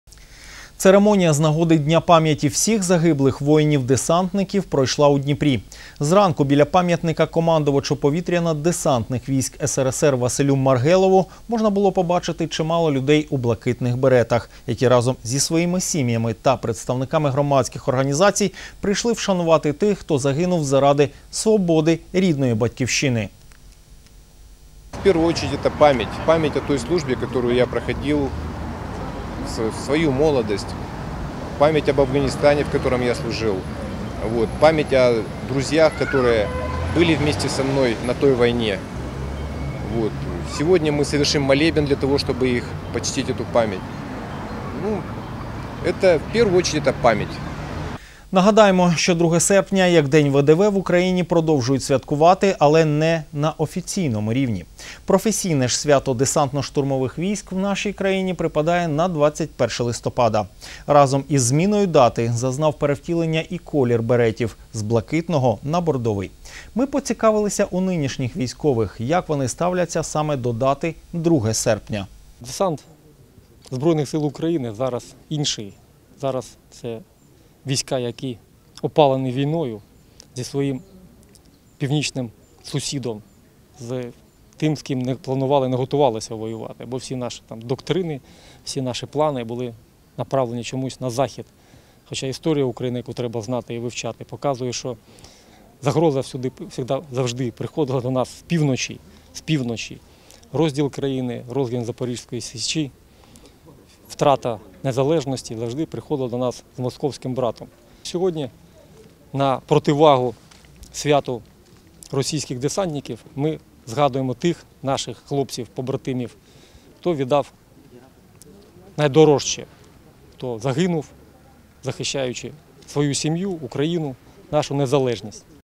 На Січеславській Набережній провели церемонію з нагоди Дня пам'яті всіх загиблих десантників.
На Січеславській Набережній провели церемонію з нагоди Дня пам'яті всіх загиблих воїнів — десантників. Пам'ятний захід провели біля пам'ятника командувачу повітряно-десантних військ СРСР Василю Маргелову. Біля монумента зібралися десятки людей у блакитних беретах. Це колишні військові, що проходили службу у повітряно-десантних військах, а також їхні сім'ї, представники громадських організацій та небайдужі Дніпряни. Хвилиною мовчання та покладанням квітів до меморіалу, усі присутні вшанували пам'ять тих, хто загинув у битвах із загарбниками, хто поклав своє життя заради свободи рідної Батьківщини.